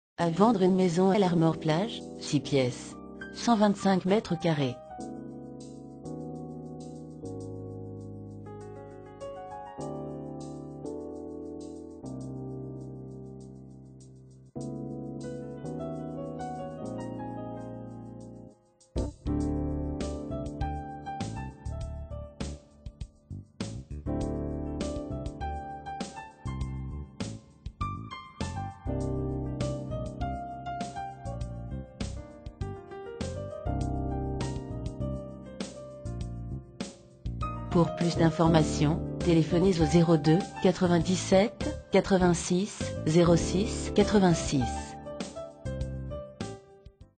Achat Vente Maison LARMOR PLAGE 56260 - 125 m2
Cliquez sur ce lien pour en savoir plus (prix, contacts)

A vendre à LARMOR PLAGE une maison de 125 m2.

L agence AVIS-Immobilier Ploemeur vous propose de nombreux autres biens immobiliers.

Annonce immobilière : Achat Vente Maison LARMOR PLAGE 56260 Morbihan